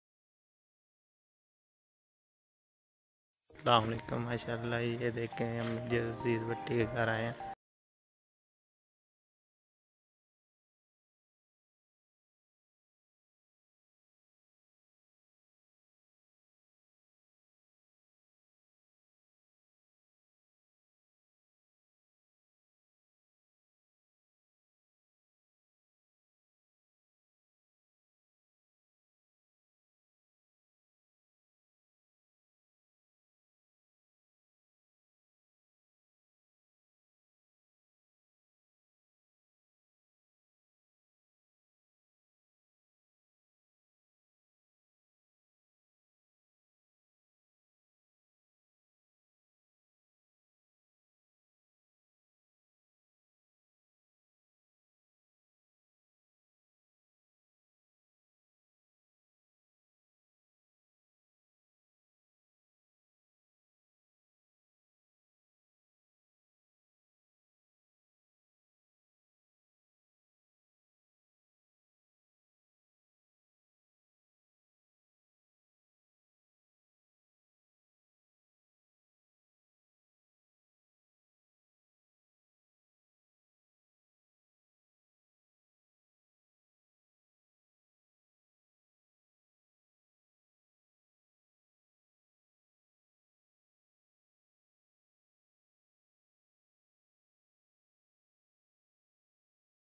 Major aziz Bhatti ki qabar kahan wakie hai??😱
A documentary on the house of aziz bhahti